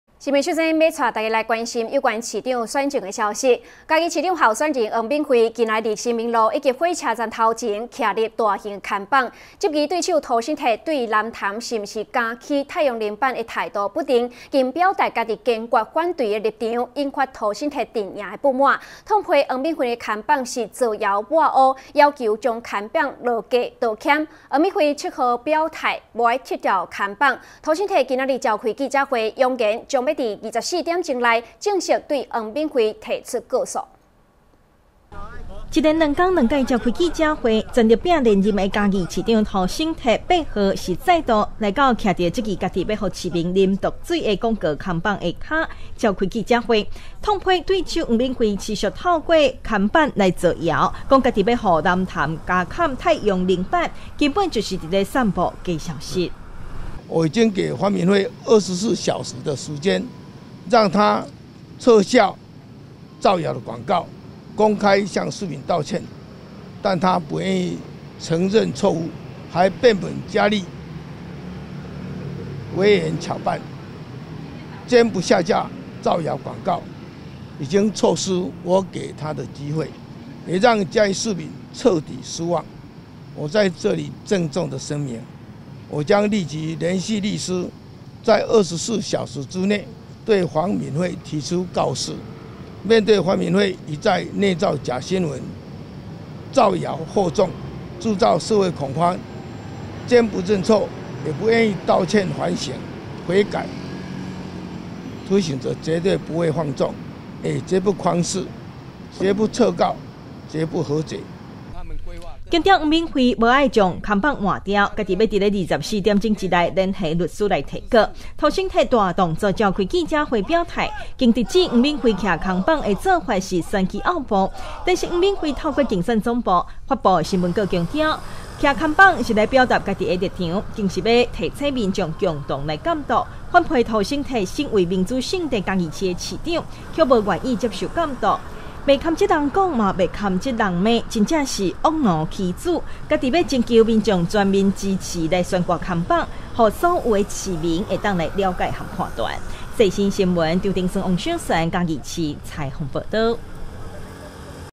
世新新聞 黃敏惠堅不撤毒水看板 涂醒哲：24小時內將提告
107年11月8日 (四) 世新新聞

黃敏惠堅不撤毒水看板 涂醒哲：24小時內將提告

嘉義市長候選人黃敏惠近來在新民路及火車站前，豎立大型看板，質疑對手涂醒哲對於蘭潭是否加蓋太陽能板的態度模糊，更表達自已堅決反對的立場，引發涂醒哲陣營不滿，痛批黃敏惠的看板是造謠抹黑，要求將看板下架、道歉，黃敏惠7日表態不撤看板，涂醒哲8日召開記者會，揚言將在24小時內，正式對黃敏惠提出告訴。